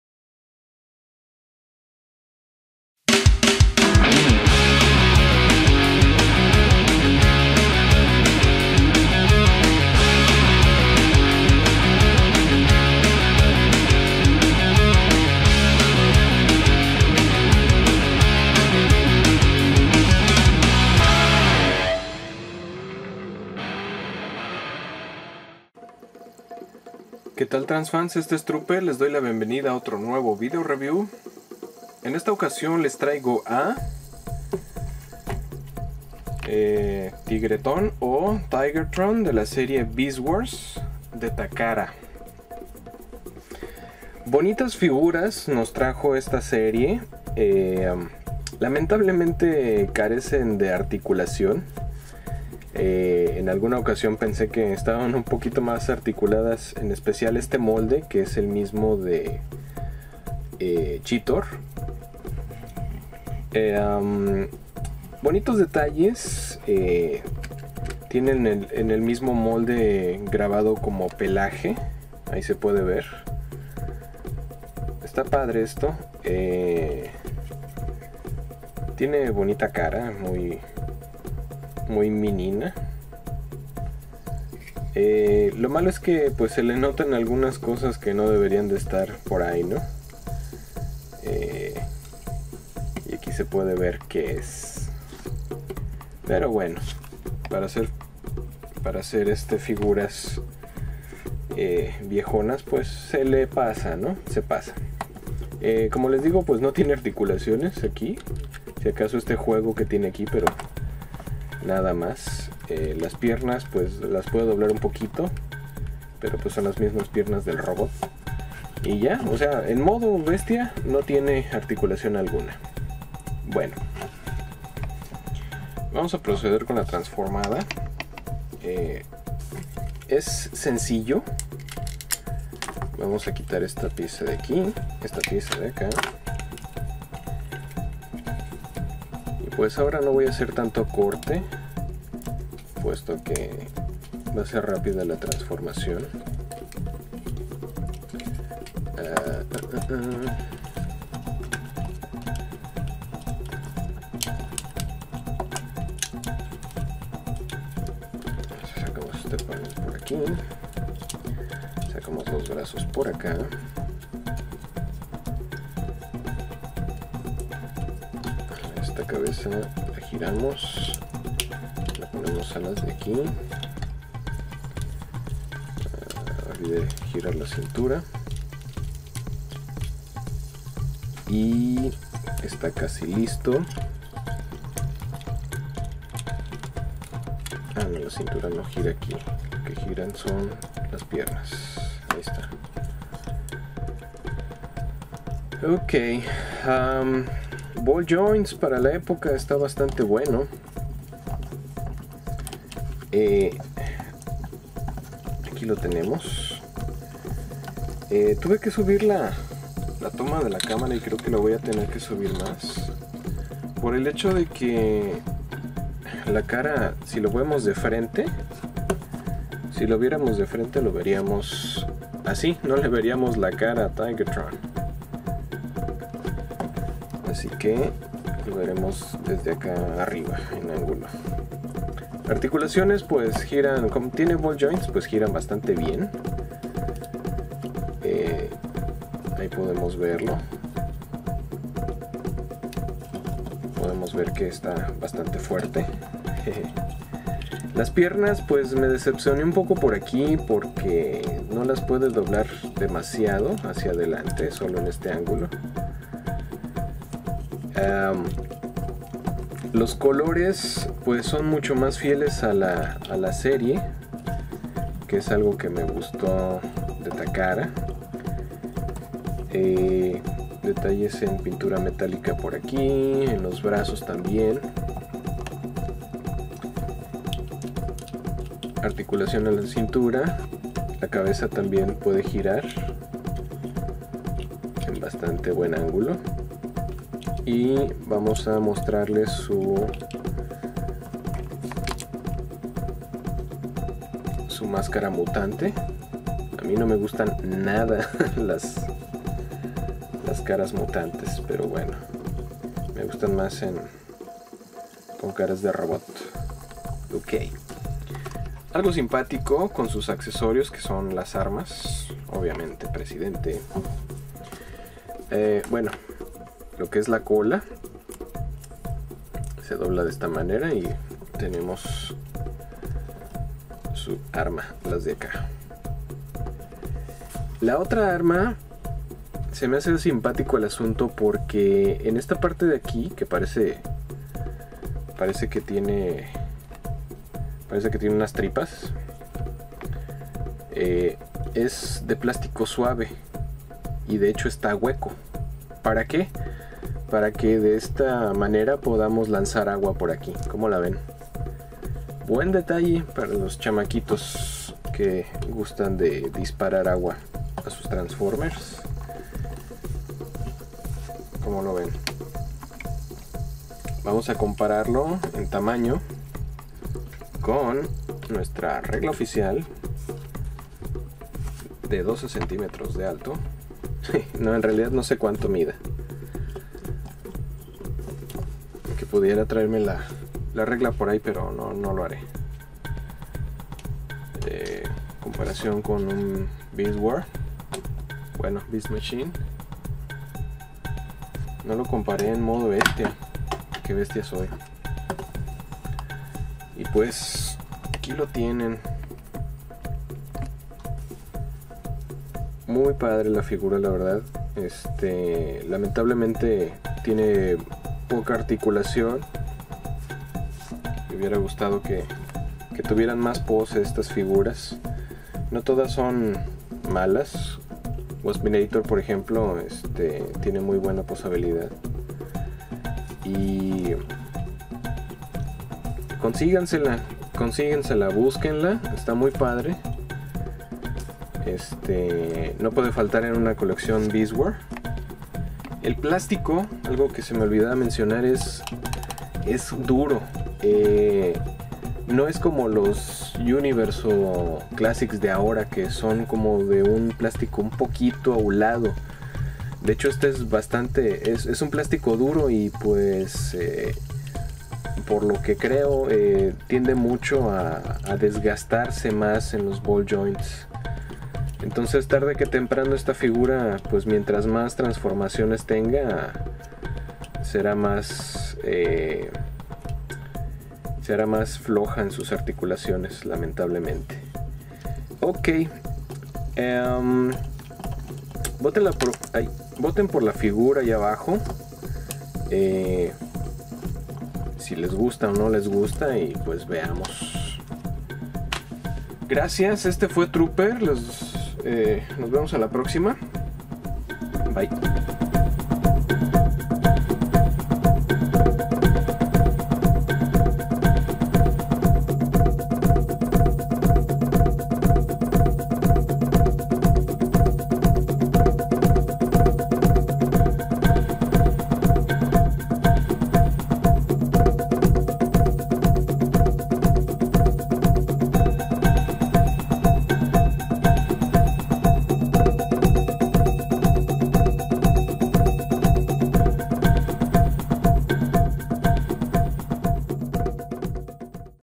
Tigatron Transformers Beast Wars Takara, revisión en español
Sin duda un personaje icónico de la serie Beast Wars Transformers, de caracter maduro y sereno, un pacifista pero cuando hay que sacar las garras las saca.
Figura de Takara con muy buen detalle y muy apegado a la serie.
Transformers Beast Wars Tigatron review.
Para ver la diferencia entre éste y el Telemocha chequen el sig.